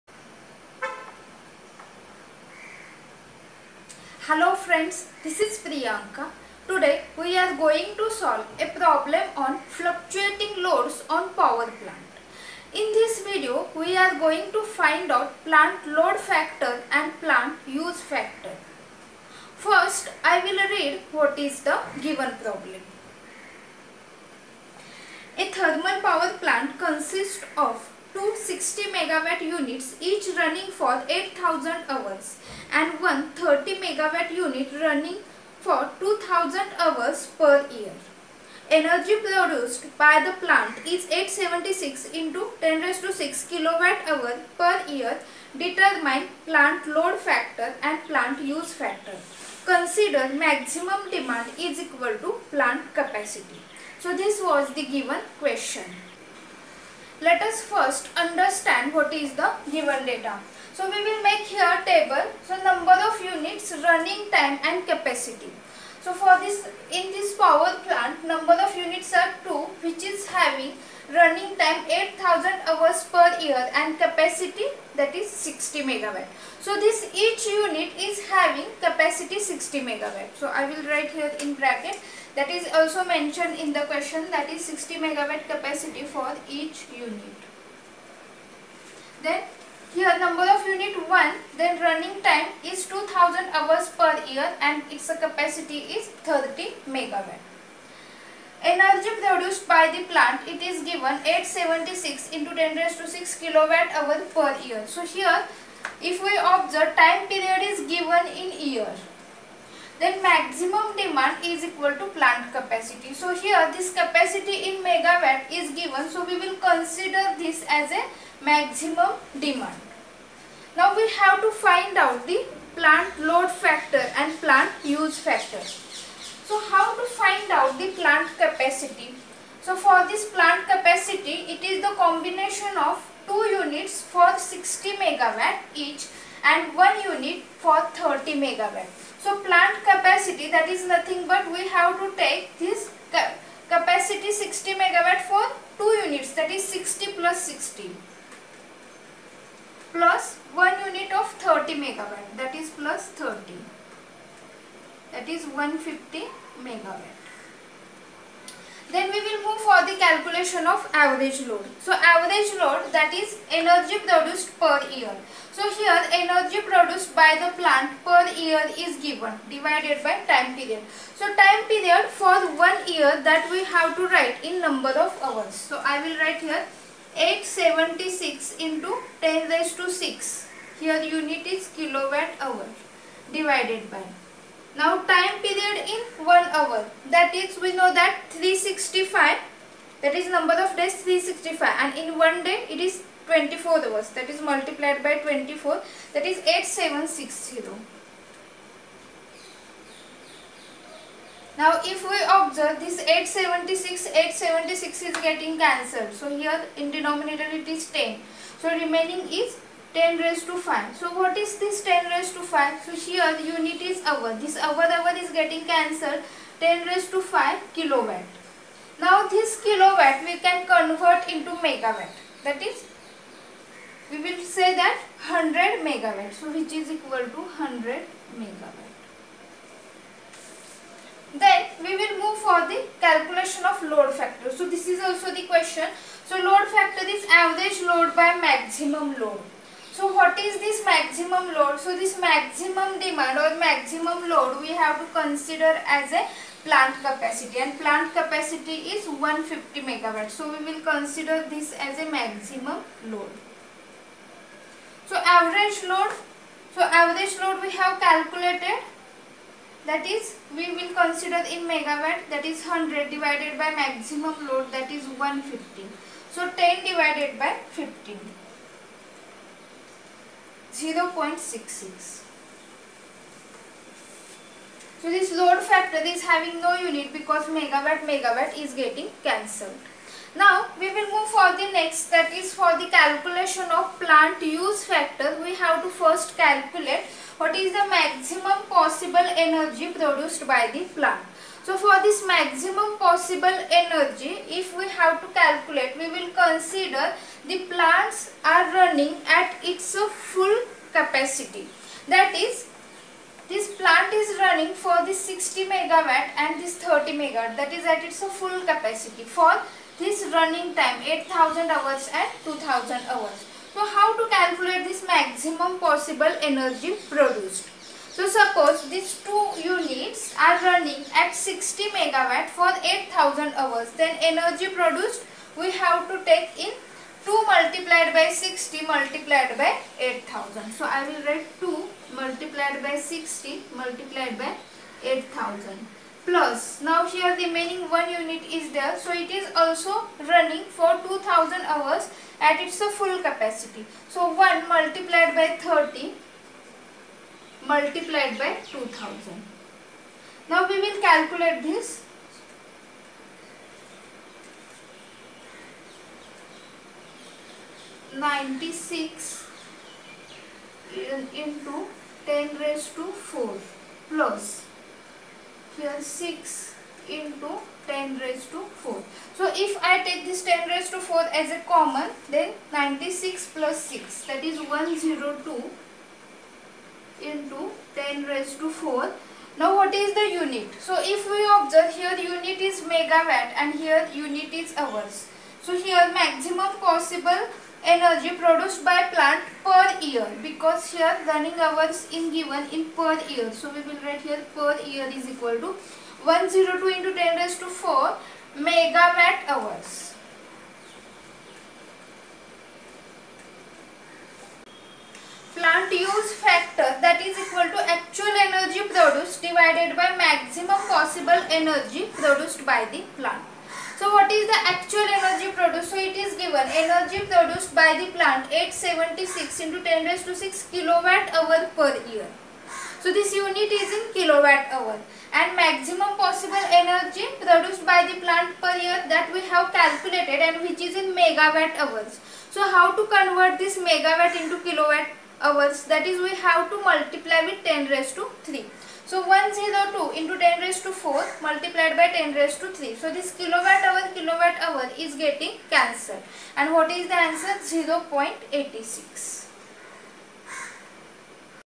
Problem 4 on plant use factor, plant load factor , Capacity factor
A thermal power plant consists of two 60 MW units each running for 8000 hours and one 30 MW unit running for 2000 hours per year. The energy produced by the plant is 876 ×10^6 kWh per year.  Determine plant load factor and plant use factor.  Consider maximum demand= plant capacity.